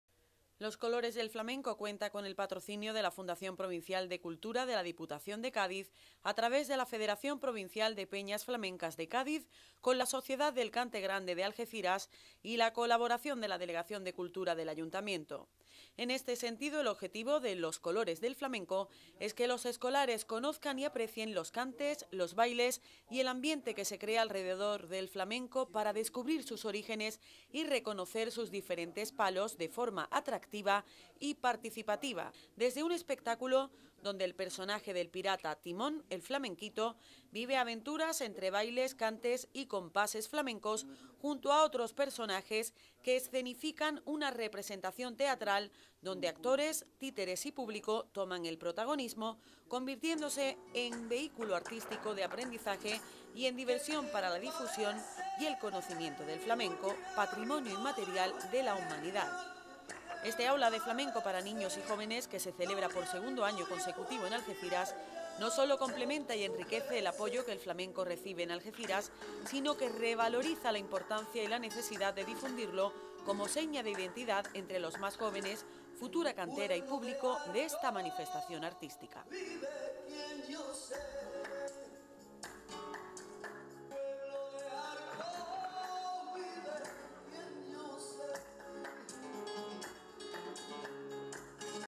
Comienza la cuarta edición del aula municipal de flamenco para niños y jóvenes
El próximo jueves, día 3 de noviembre, a las once de la mañana en el Museo Municipal, comenzará la cuarta edición del Aula de Flamenco para niños y jóvenes “Los colores del Flamenco”, un evento artístico, flamenco y docente, con la presencia de varios centros escolares de la ciudad, que participarán en este programa, una inicitiva  que pretende acercar el flamenco a los más jóvenes, de forma didáctica y atractiva.